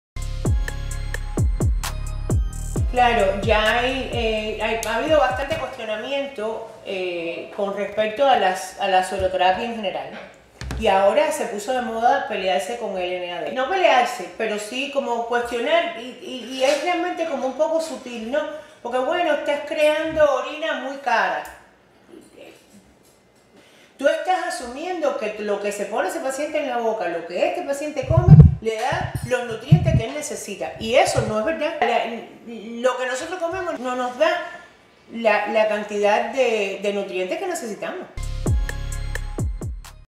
Sueroterapia y NAD - Medicina Regenerativa
La Dra. Maritza Novas nos habla un poco de la sueroterapia y el NAD.